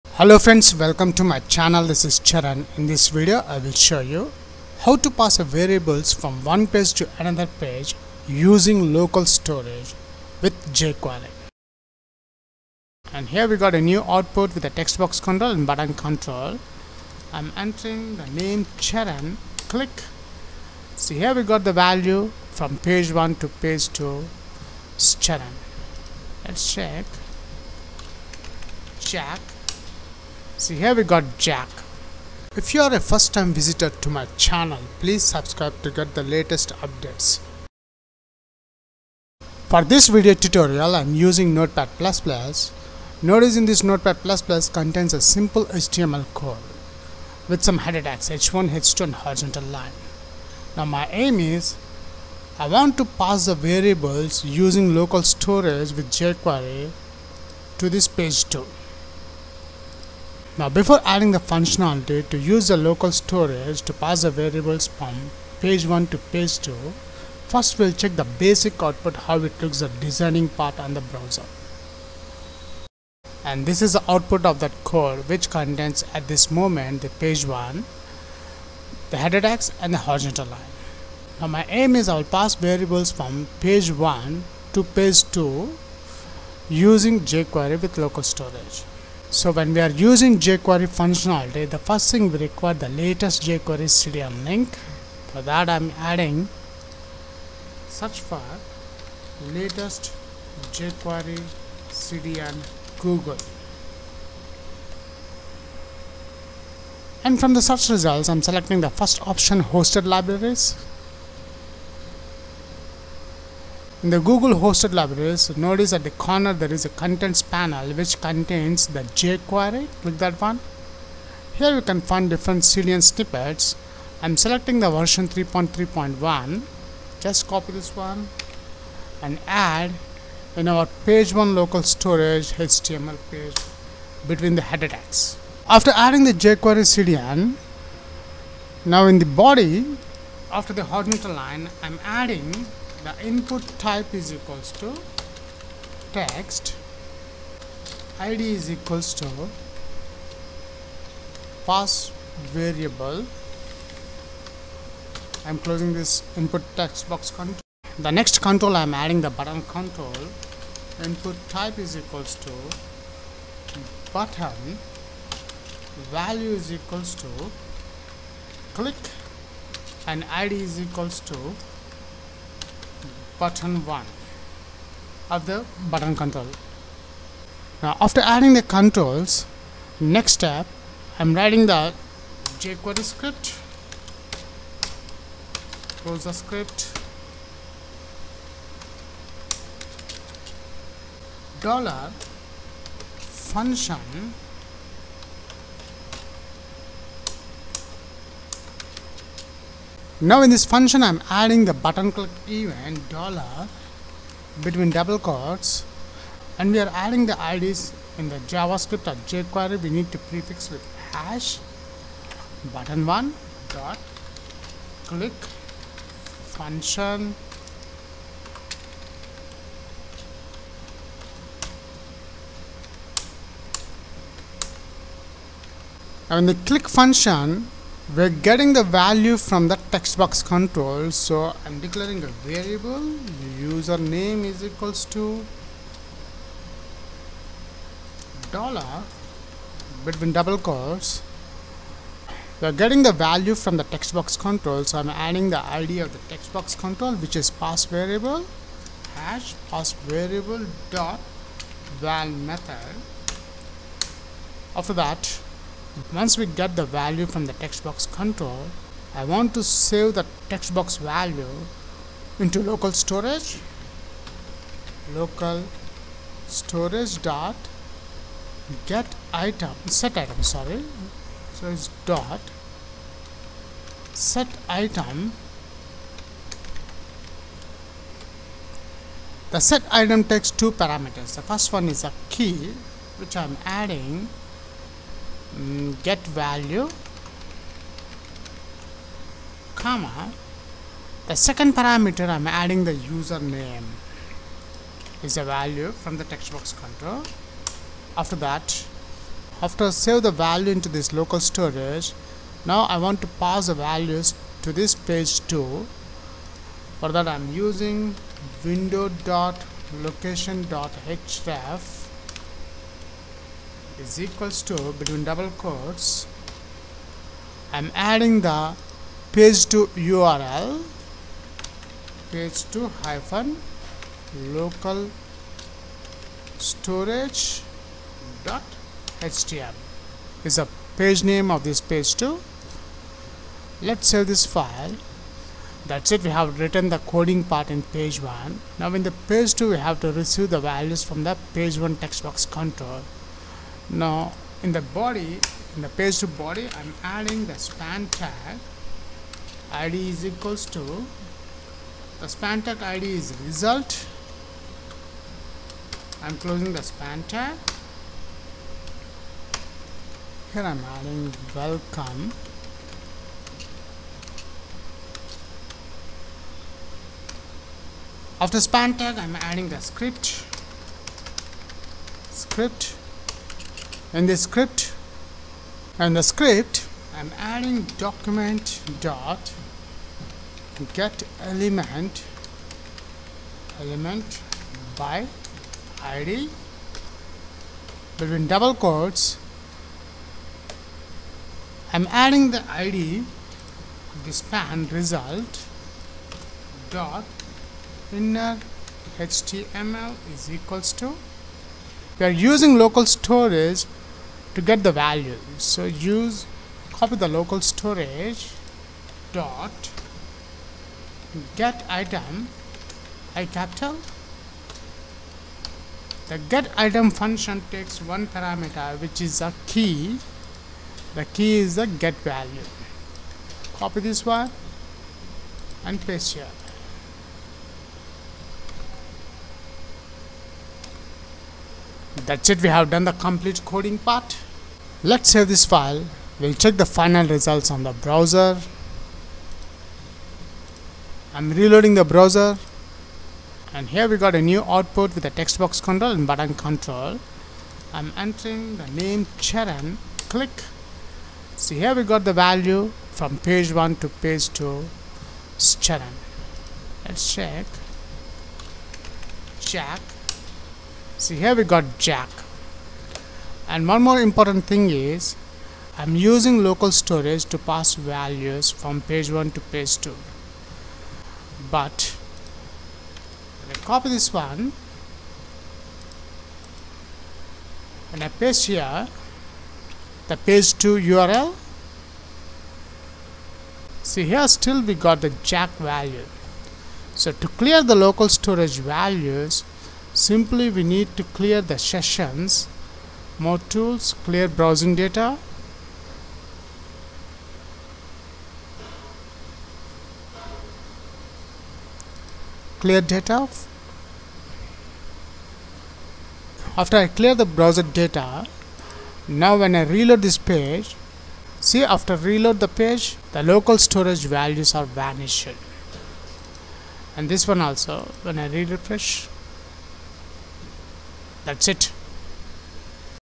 html local storage example with jquery 3.3.1
local storage html with jquery pass variables from one page to another page